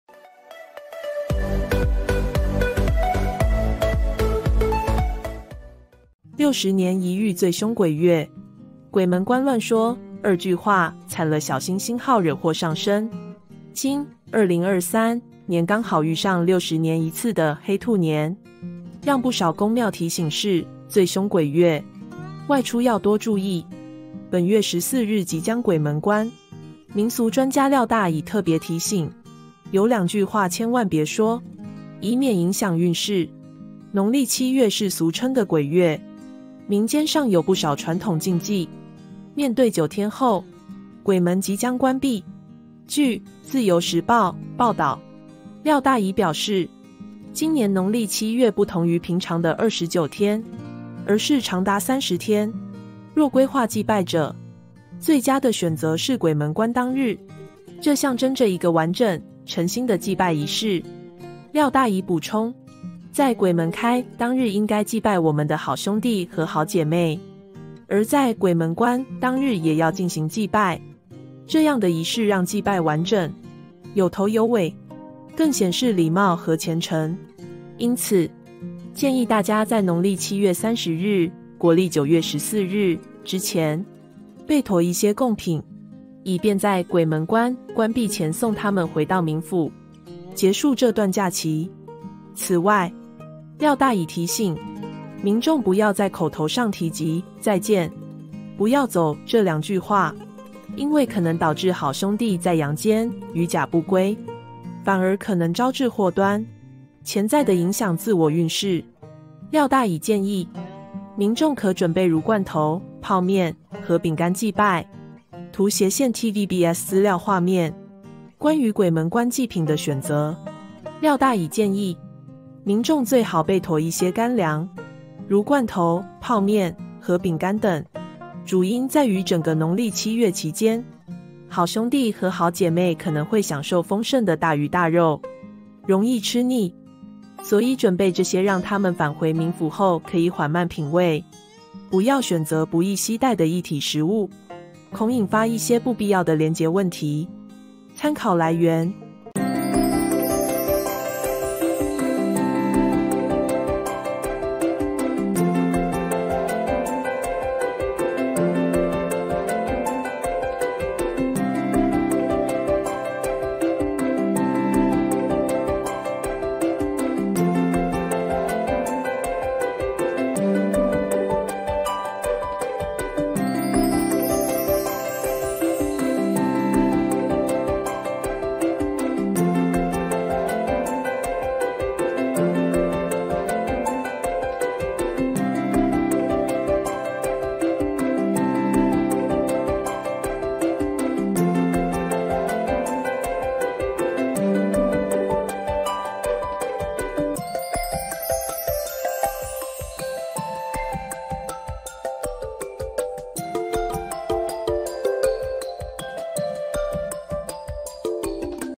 60年一遇最兇鬼月！鬼門關亂說「2句話」慘了 小心%2A惹禍上身
今（2023）年剛好遇上60年一次的「黑兔年」，讓不少宮廟提醒是「最兇鬼月」，外出要多注意。本月14日即將鬼門關，民俗專家廖大乙特別提醒，有兩句話千萬別說，以免影響運勢。

農曆7月是俗稱的鬼月，民間上有不少傳統禁忌。面對9天後，鬼門即將關閉，據《自由時報》報導，廖大乙表示，今年農曆7月不同於平常的29天，而是長達30天。若規劃祭拜者，最佳的選擇是鬼門關當日，這象徵著一個完整、誠心的祭拜儀式。傳遞正能量
Give me a Like when you are free 🙏